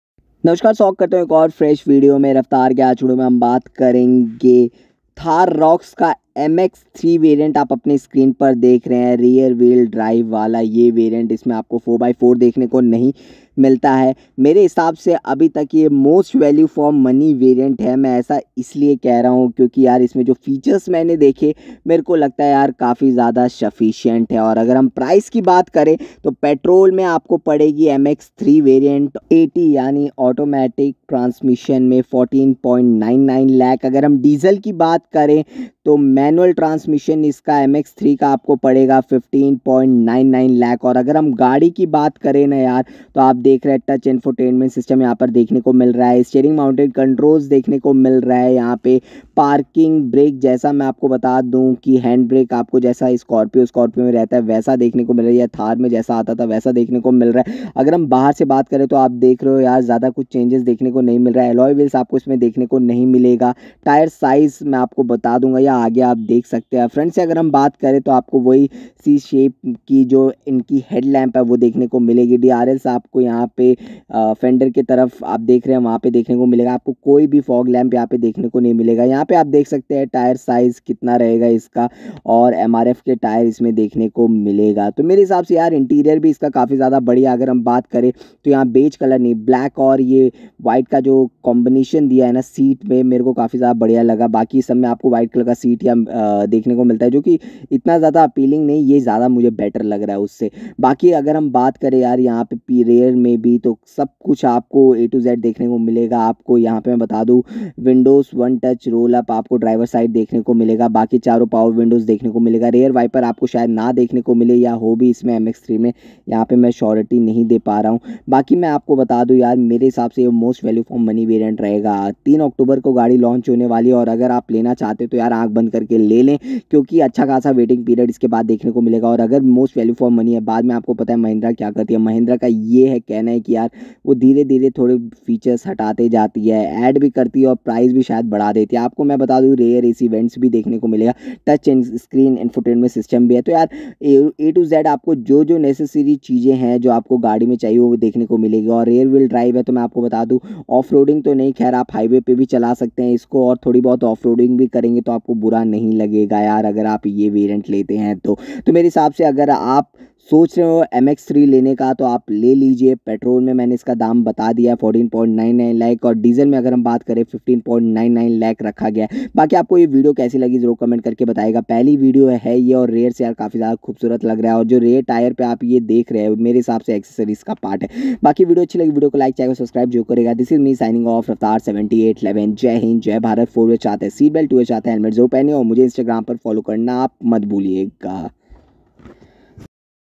Mahindra Thar Roxx MX 3 AT Most Value For Money variant walkaround video Just 14.99 Lakh
mx3 variant is most value for money variant in Thar roxx you get all the basic features which needed in a SUV it comes with steering mounted controls infotainment system.